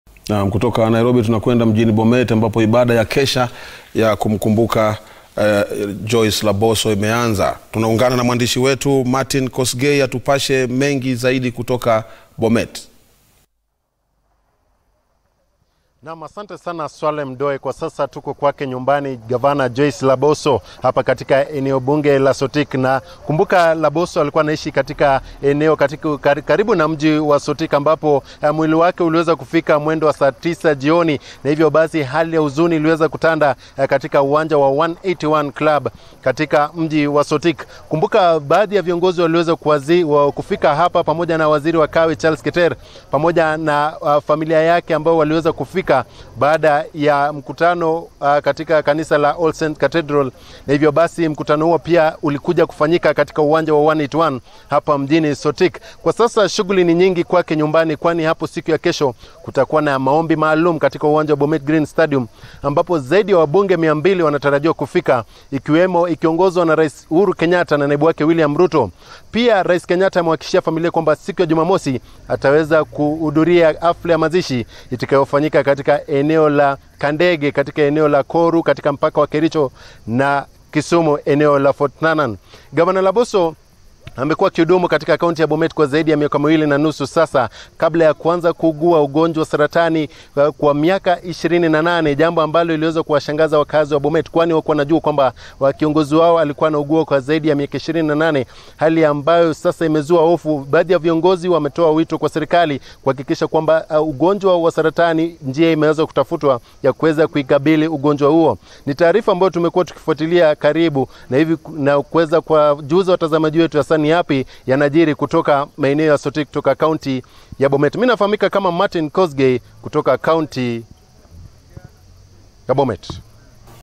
Kesha ya Joyce Laboso yafanyika Bomet
Na mjini Bomet ibada ya kesha ya kumkumbuka aliyekuwa gavana wa kaunti hiyo marehemu Joyce Laboso imeanza. mwandishi wetu Martin Kosgey anafuatilia ibada hiyo na sasa tunaungana naye moja kwa moja atupashe zaid